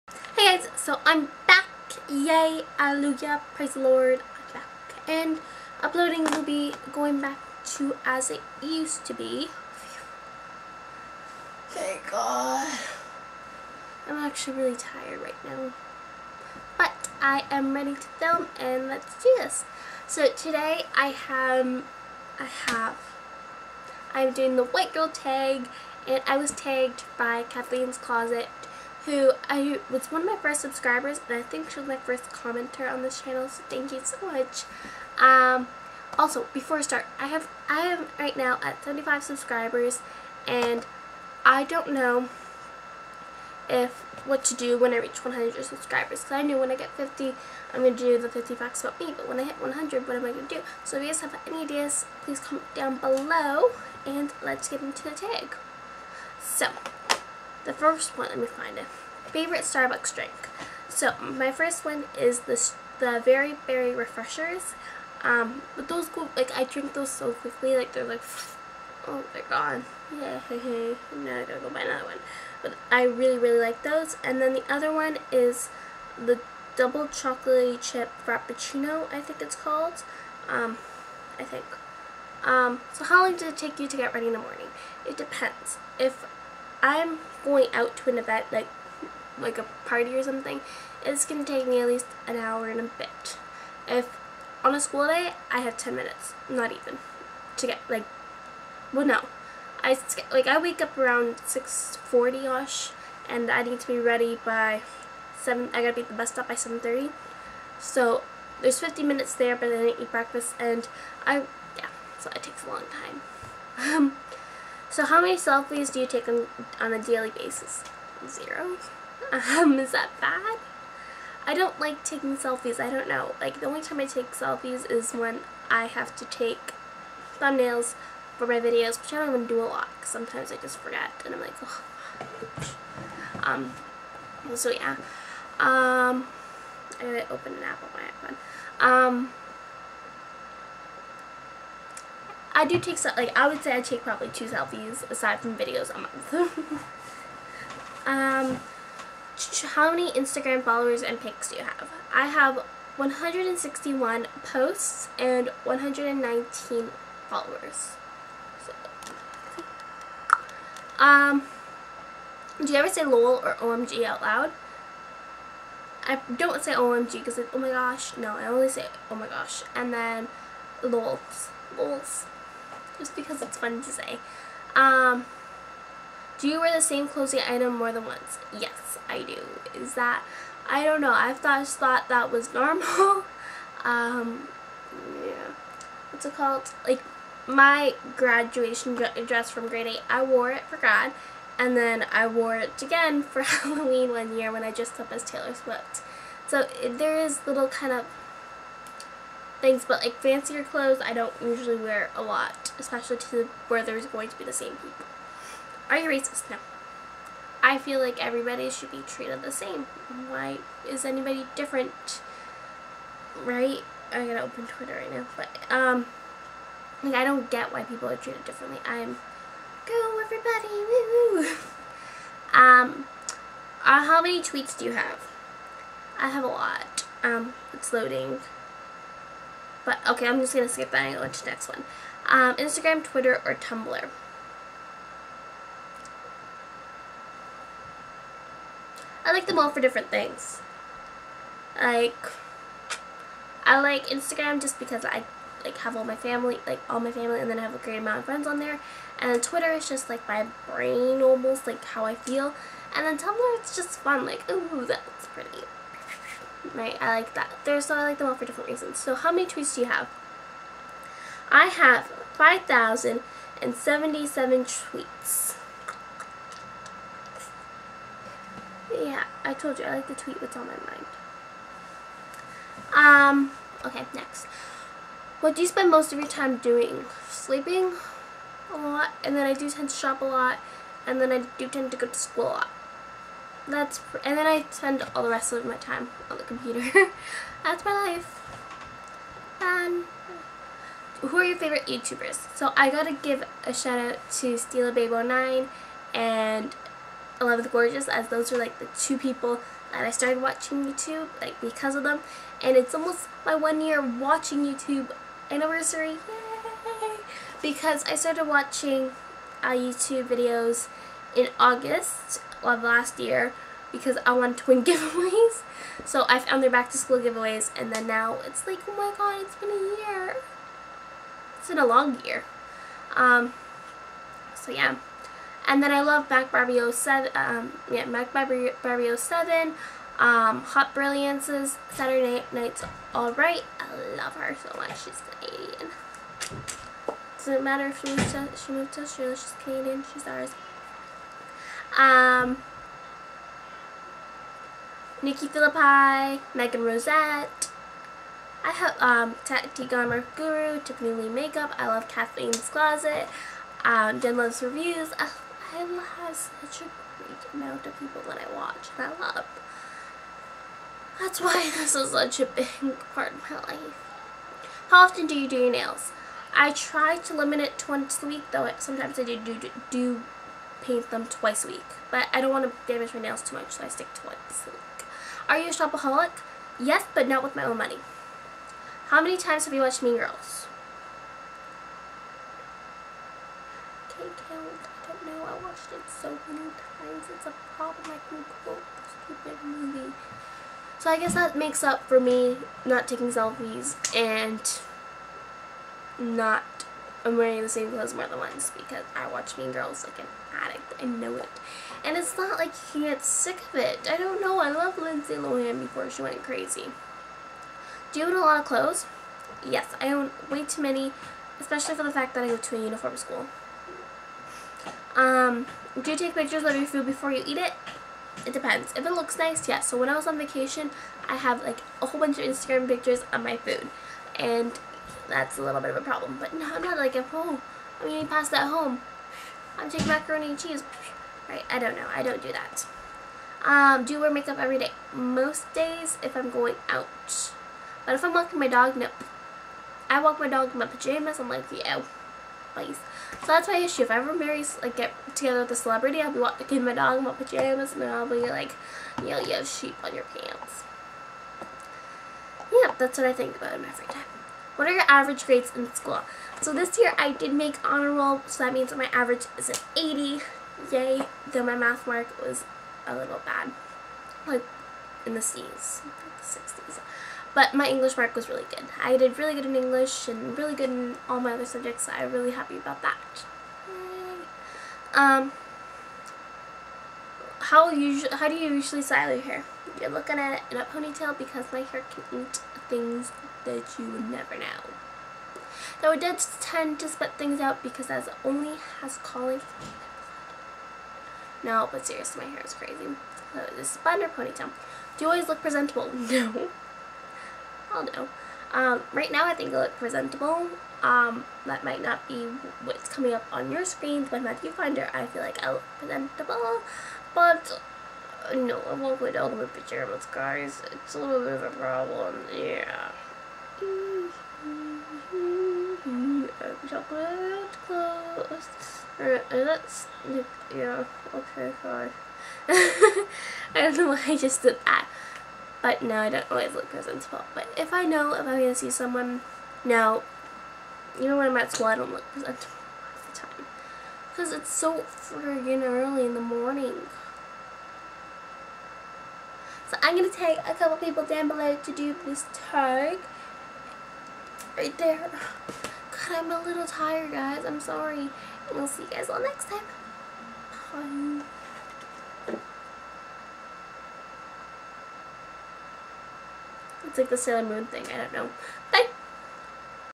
White Girl Tag!!!
Questions
1. Favorite starbucks drink?
2. How long does it take you to get ready in the morning?
3. How many selfies do you take on a daily basis?
4. How many IG followers and pics do you have?
5. Do you ever say "LOL" or "OMG" out loud?
6. Do you wear the same clothing item more than once?
7. Are you racists?
8. How many tweets do you have?
9. Instagram, Twitter, or Tumblr?
10. What do you spend most of your time doing?
11. Who are your favorite youtubers?
12. How often do you do your nails?
13. Are you a shopaholic?
14. How many times have you watched Mean Girls?
15. Do you own a lot of clothes?
16. Do you take pictures of your food before you eat it?
17. Do you wear makeup everyday?
18. What are your average grades in school?
19. How do you usually style your hair?
20. Do you always look presentable?

My Links!!!